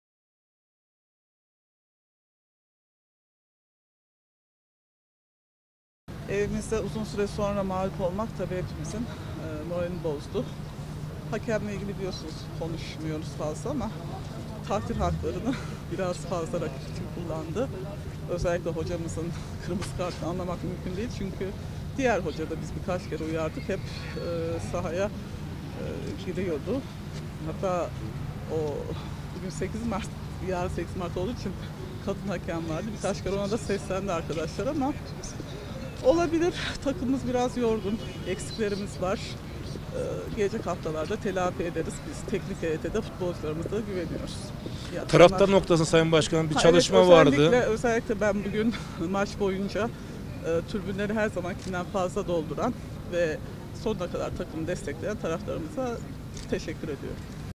Berna Gözbaşı: "Hakem takdir haklarını hep Alanyaspor'dan yana kullandı"
GÖRÜNTÜ DÖKÜMÜ :
- Yukatel Kayserispor Kulübü Başkanı Berna  Gözbaşı'nın konuşması KAYSERİ - Spor Toto Süper Lig'in 28. haftasında Aytemiz Alanyaspor, konuk olduğu Yukatel Kayserispor'u 2-1 yendi. Maçın ardından Yukatel Kayserispor Kulübü Başkanı Berna Gözbaşı açıklamalarda bulundu.